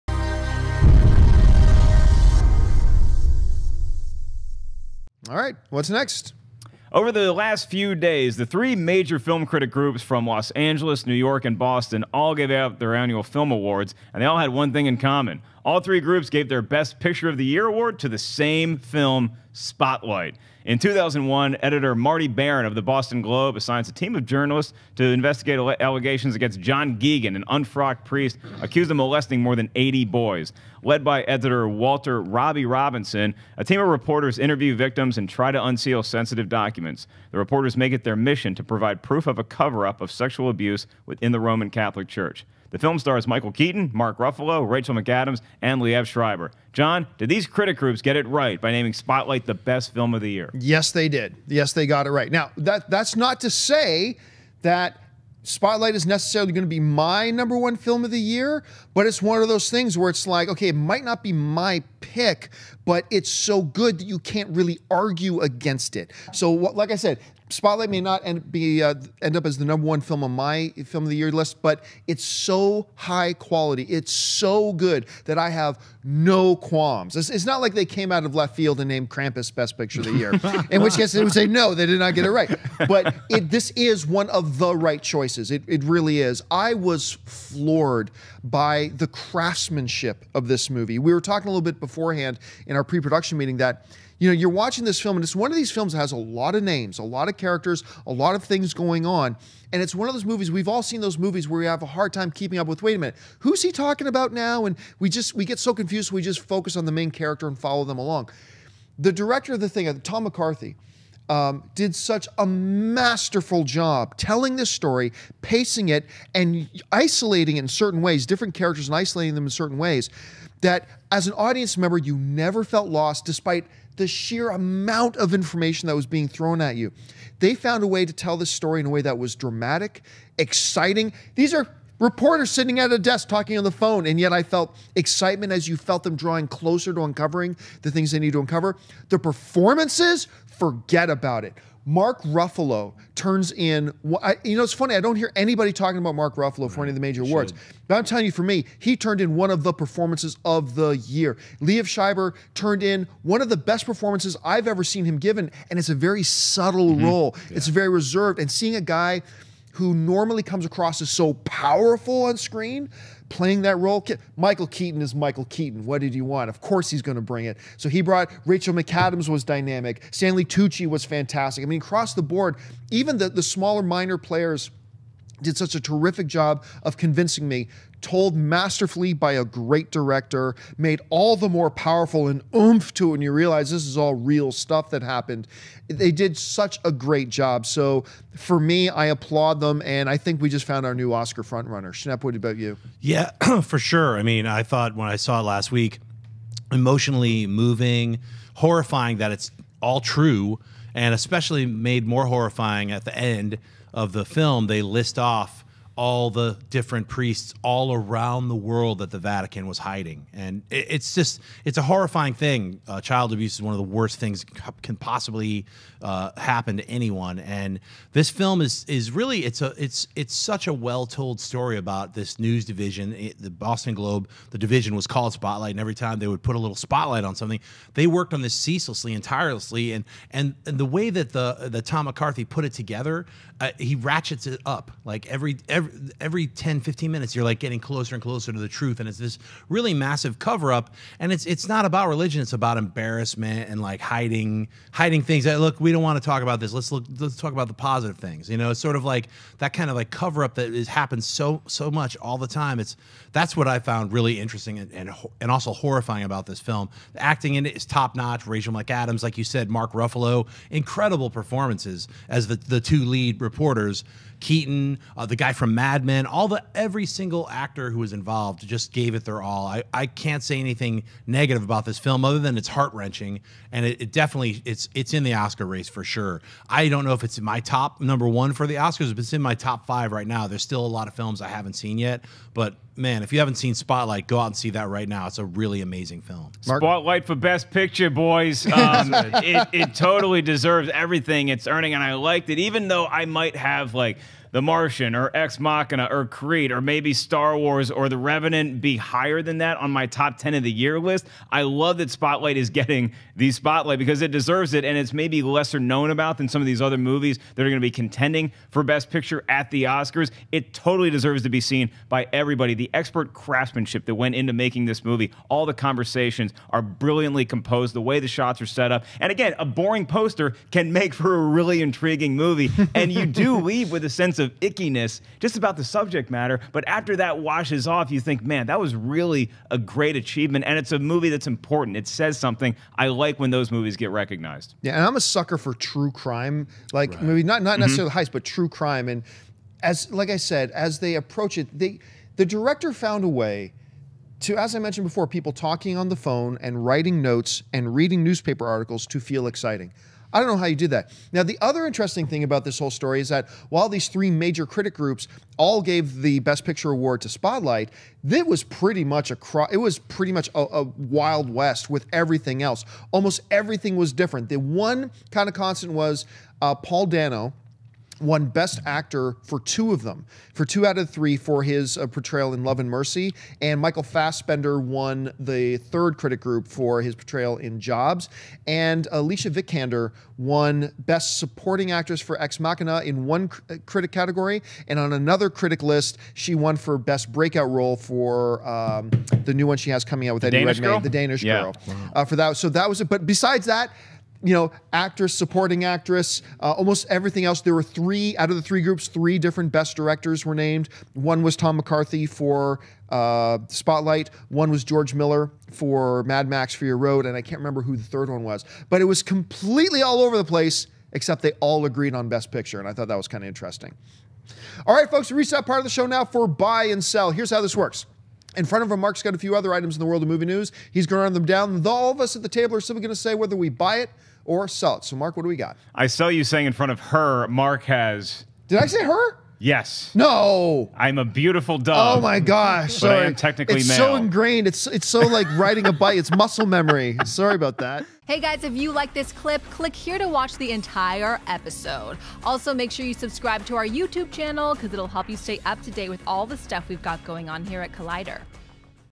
New York, Los Angeles, and Boston Critics give Spotlight Best Picture - Collider
Over the last few days the 3 major film critic groups from Los Angeles, New York and Boston, all gave out their annual film awards, and they all had one thing in common. All three groups gave their BEST PICTURE of the year award to the same film. SPOTLIGHT. In 2001, editor Marty Baron of The Boston Globe assigns a team of journalists to investigate allegations against John Geoghan, an unfrocked priest accused of molesting more than 80 boys. Led by editor Walter "Robby" Robinson, a team of reporters interview victims and try to unseal sensitive documents. The reporters make it their mission to provide proof of a cover-up of sexual abuse within the Roman Catholic Church. The film stars Michael Keaton, Mark Ruffalo, Rachel McAdams and Liev Schreiber.